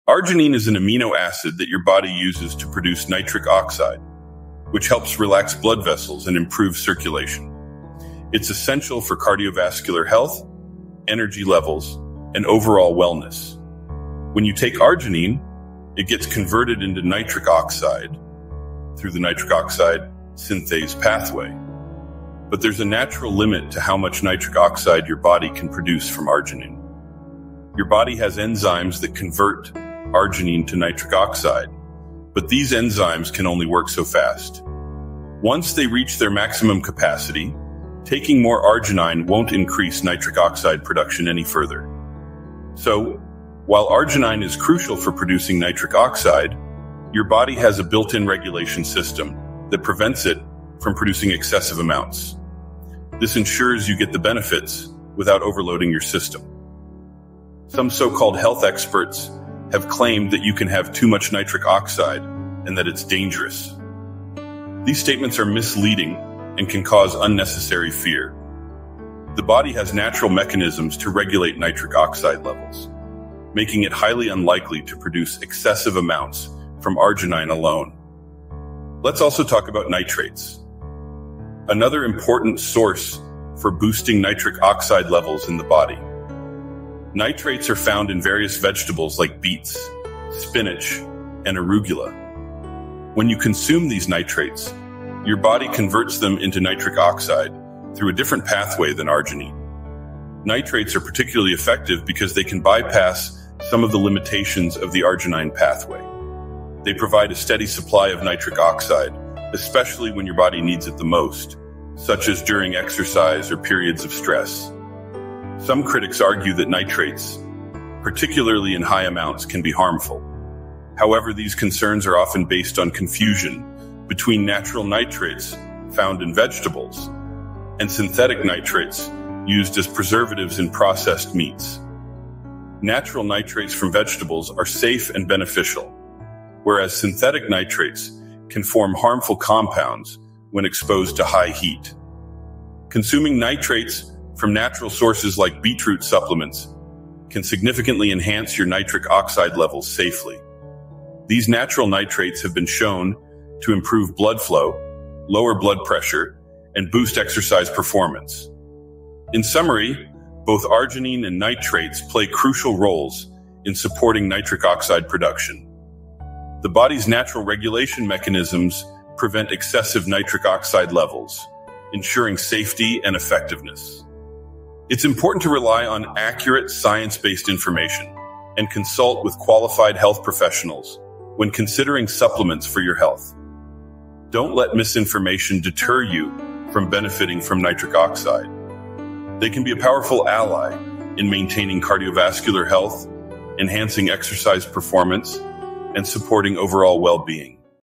Can you take too much nitric oxide?
Arginine is an amino acid that your body uses to produce nitric oxide, which helps relax blood vessels and improve circulation. It's essential for cardiovascular health, energy levels, and overall wellness.

When you take arginine, it gets converted into nitric oxide through the nitric oxide synthase pathway. But there's a natural limit to how much nitric oxide your body can produce from arginine. Your body has enzymes that convert arginine to nitric oxide, but these enzymes can only work so fast. Once they reach their maximum capacity, taking more arginine won't increase nitric oxide production any further.